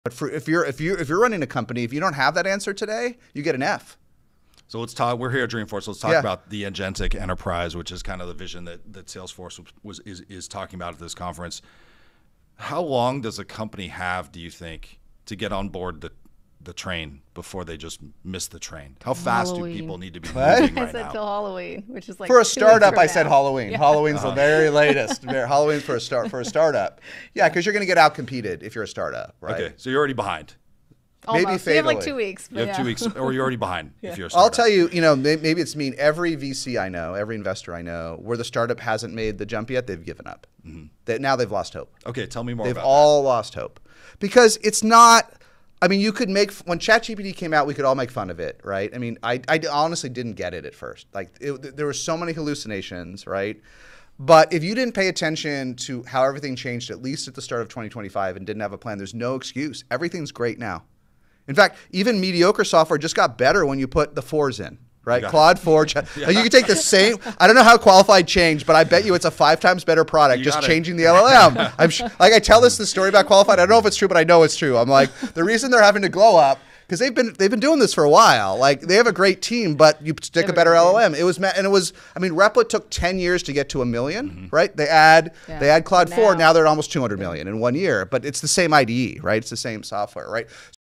Agentic Enterprise: The Urgent Deadline for Companies!
Startups must integrate AI by Halloween or risk being outcompeted. Investors are losing hope for those who haven't adapted. Even mediocre software improves with advanced LLMs. Replit's growth skyrocketed after adding Claude 4.0.